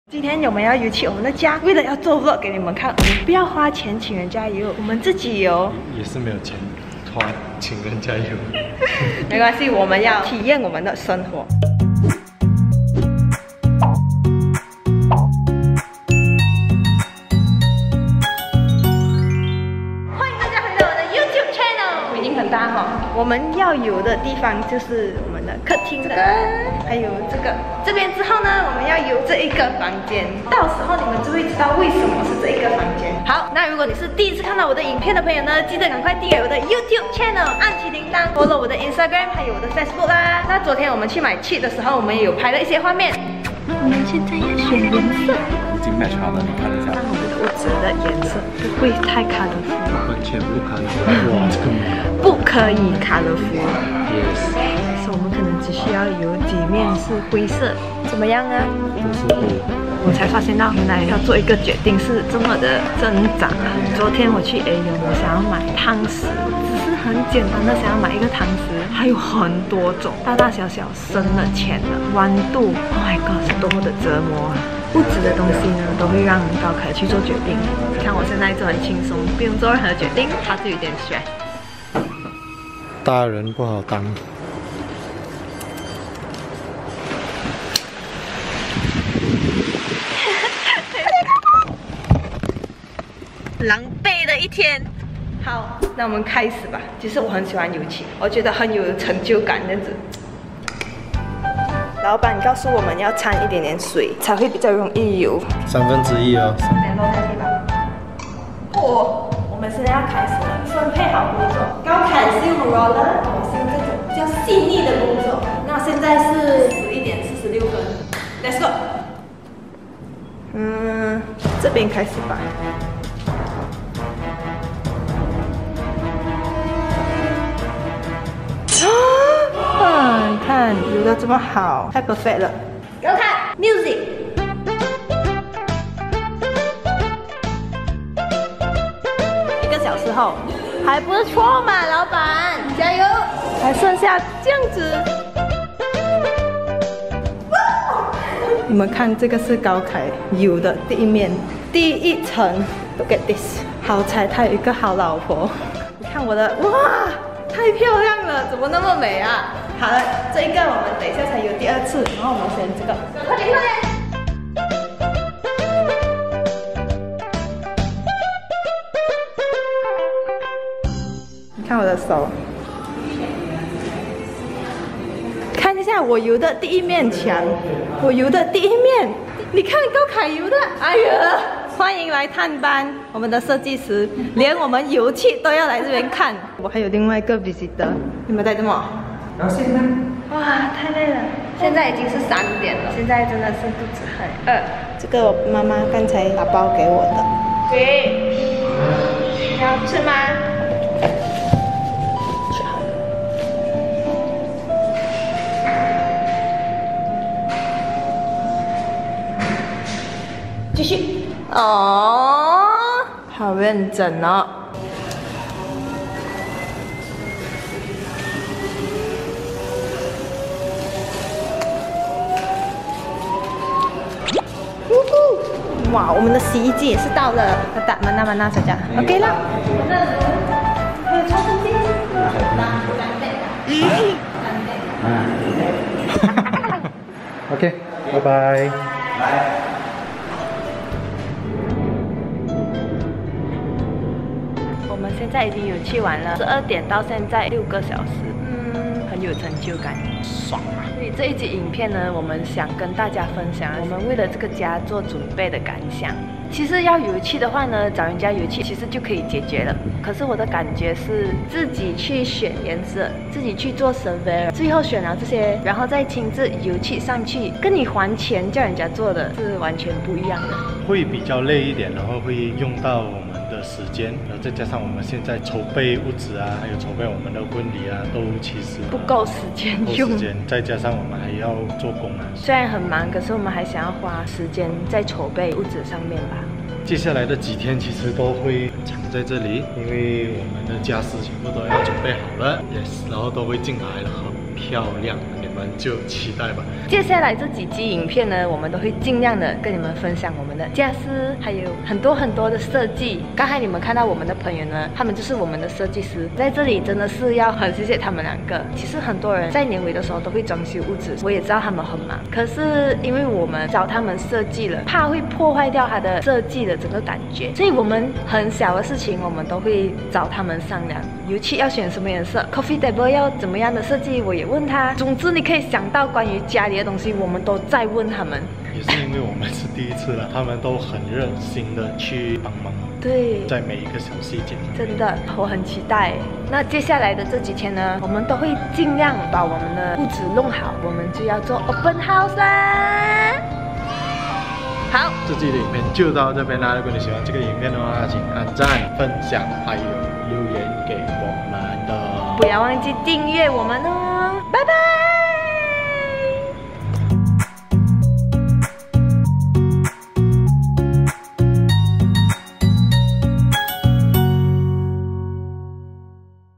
「挑战油漆DIY」还敢说男女平等吗！😝 到底是男人还是女人比较厉害呀？！【CC字幕】
距离我们上一个介绍屋子的影片已有一段时间啦！现在让你们看看那一些些的进度吧～

希望我的每一部影片，都会带给大家一些些的快乐。

高凯求婚的影片
（一）回忆篇